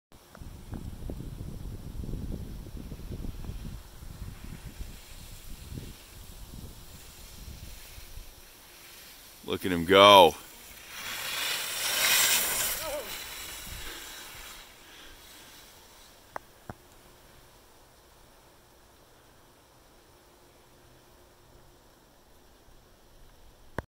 Asnes Ingstad on the east coast hardpack
.aaaaannnd I almost, almost, wipe out.
Asnes Ingstad 195cm (wax)
Rottefella Super Telemark binding
Alpina Alaska 75mm

In this video I'm leaning into the turn a bit here with the lurk. I usually counter-rotate in the opposite direction with the lurk, which provides better balance and sets me up for the next turn.